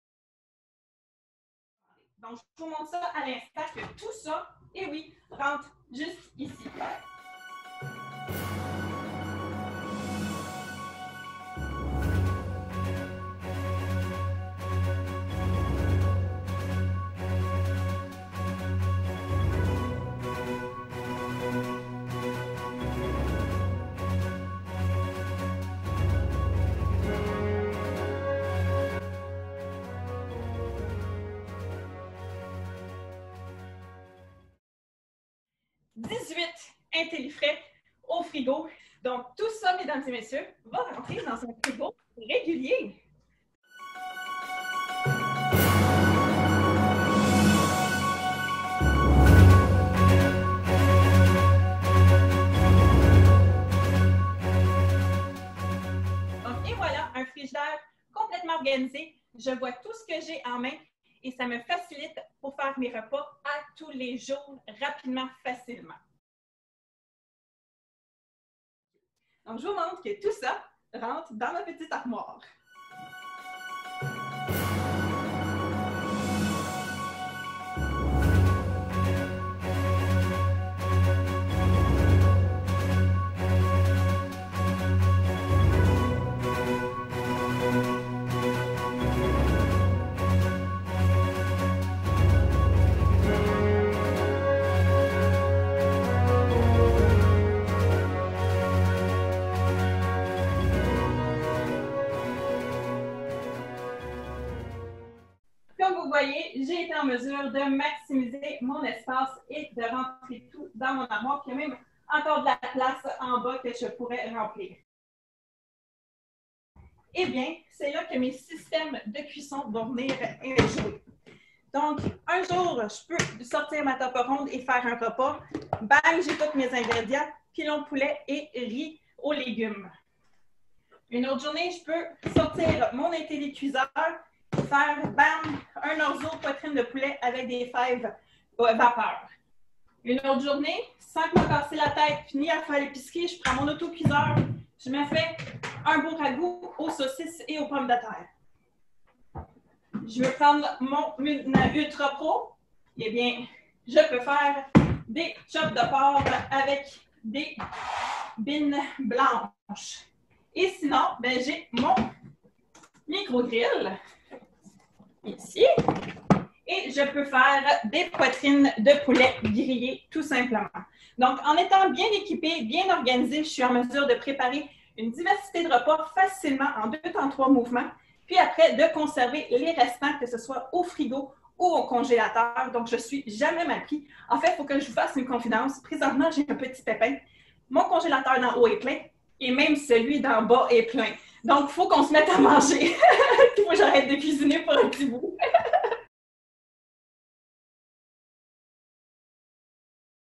Ça rentre! Systèmes d'organisation avec Mélissa Leclerc
On montre combien on peut jouer à Tétris facilement avec nos systèmes d'organisation !